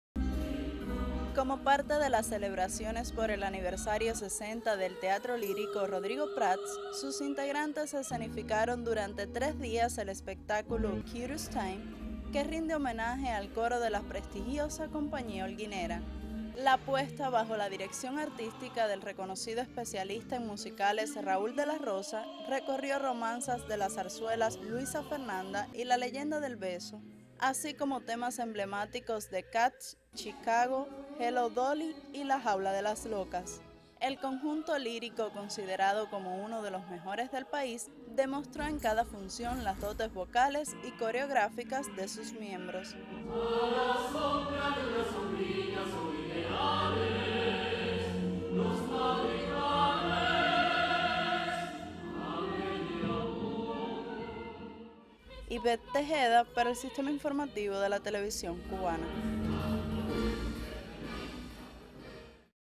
Teatro lírico Rodrigo Prats realizó presentaciones como parte de la celebración por su aniversario
Periodista: Ivett Tejeda